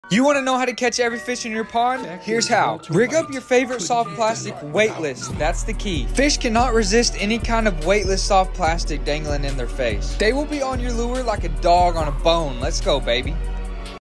Catch EVERY pond bass!!!
I am obsessed with throwing weightless senkos. They work in almost all conditions when bass fishing. You can catch every bass in the pond and they are super affordable.